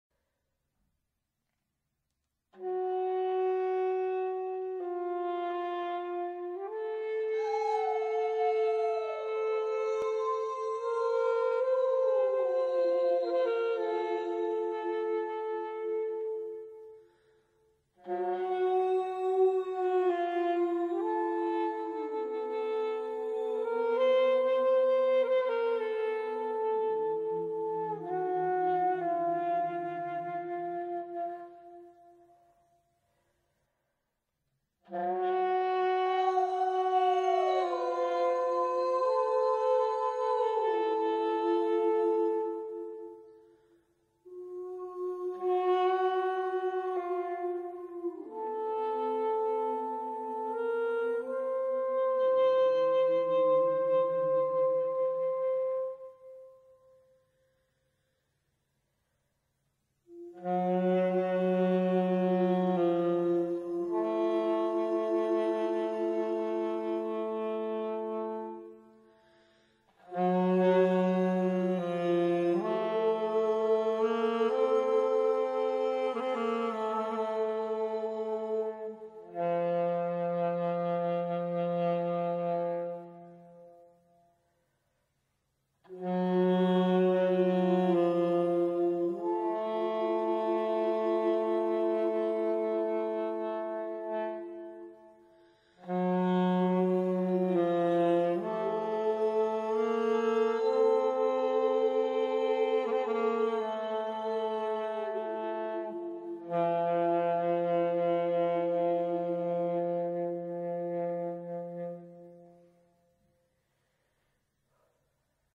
Ulvoo saxophone 03112019 Holopainen
I played my saxophone, when my friend song a duet with my dog.